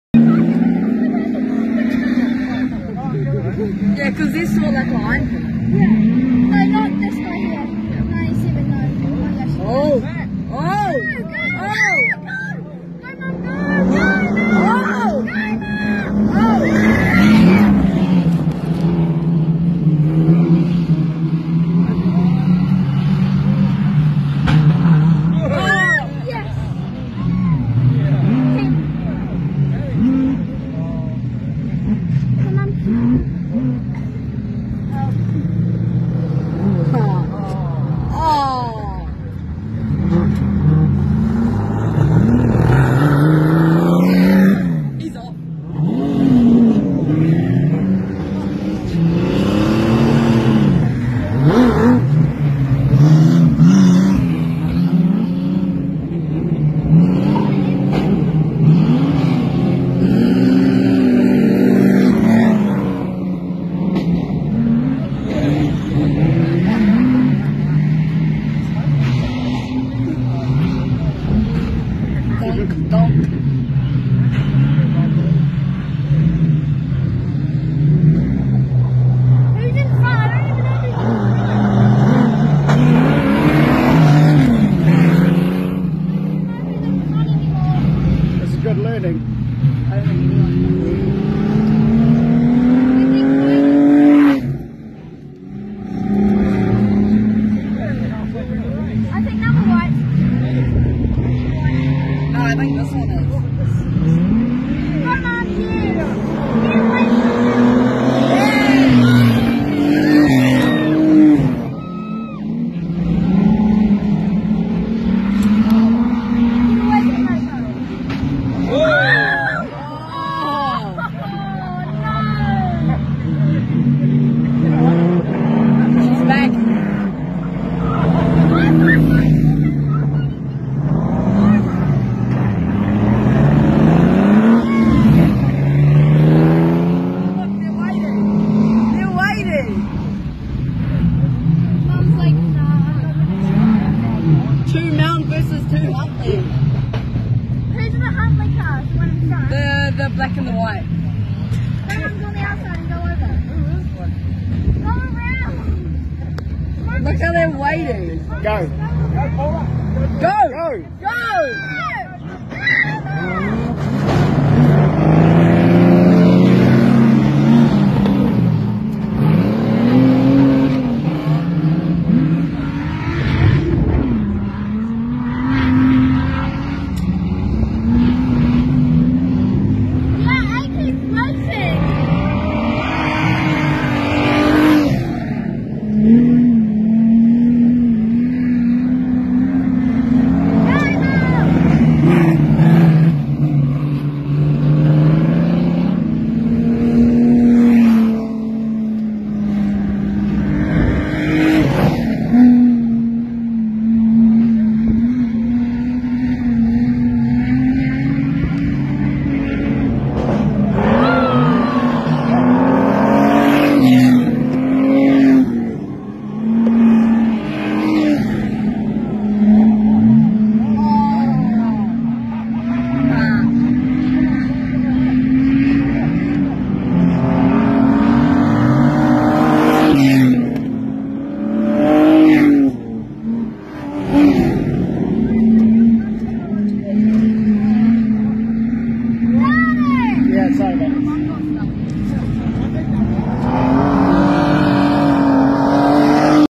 Super Stockcar Race at Waihi Beach Dirt Track
Waihi Beach Dirt Track Club - Stockcar Race Part 2
We are a group of like minded people who just love to race on dirt! • We believe in racing with as sense of fair play and sportsmanship